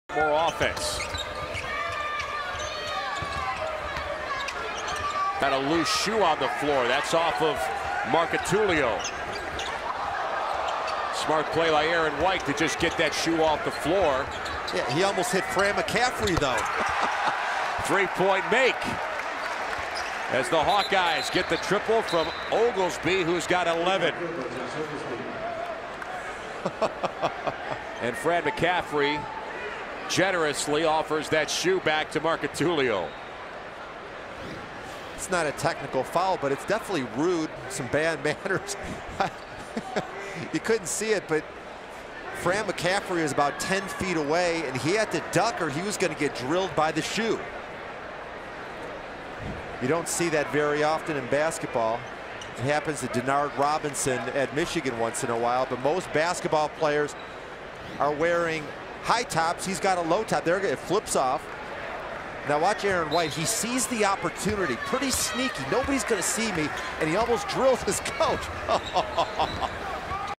Who Throws a Shoe?
Iowa head coach Fran McCaffery has to duck a flying shoe during Iowa's game at Northwestern on Sunday.